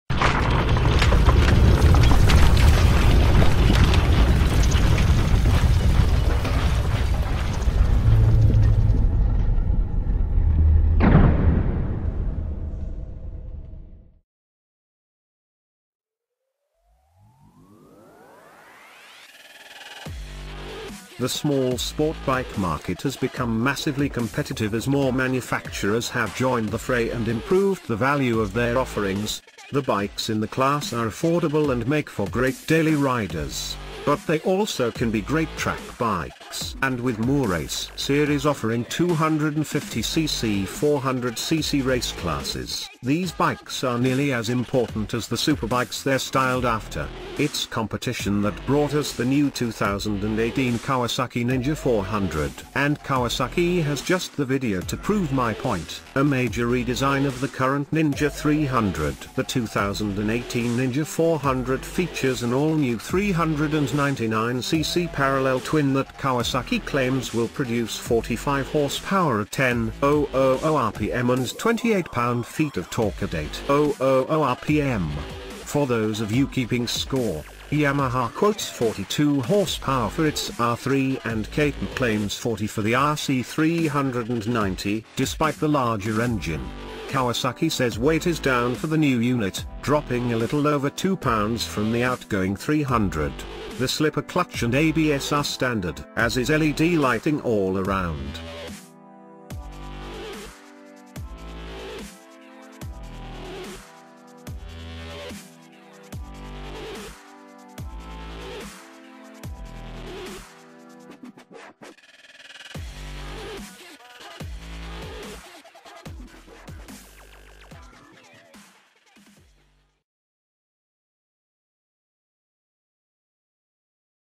AWESOME !!!2018 new Kawasaki Z400 revealed model Price & Spec
AWESOME !!!2018 new Kawasaki Z400 revealed model Price & Spec - The small sportbike market has become massively competitive as more manufacturers have joined the fray and improved the value of their offerings. The bikes in the class are affordable and make for great daily riders, but they also can be great trackbikes. And with more race series offering 250cc–400cc race classes, these bikes are nearly as important as the superbikes they're styled after.

██████ GET DAILY UPDATES! NOW! ██████